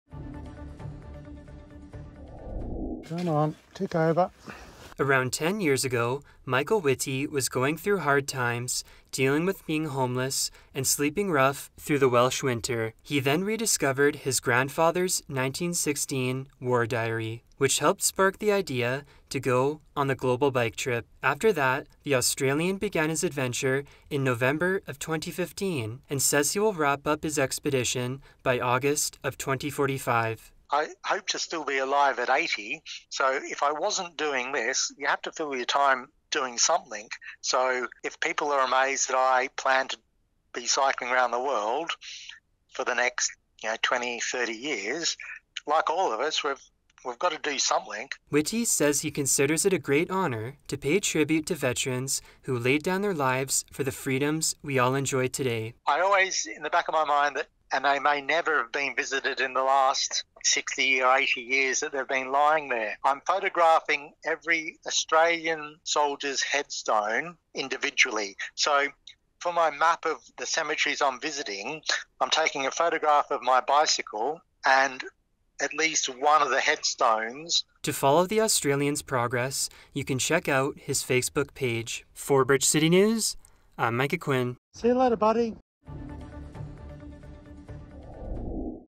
Man Cycling Around The Planet To View War Graves | July 11, 2023 | Micah Quinn | Bridge City News
An Australian man is doing the near impossible and all through the power of his bike. Michael Whitty has embarked on a cross-Canada journey to visit Commonwealth War Grave sites across our country. What’s even more spectacular is that he has a plan to check out 25,630 war grave locations around the world. BCN’s Micah Quinn caught up with Whitty near Bow Island, Alberta to hear his incredible story.

Aired on: Tuesday, July 11, 2023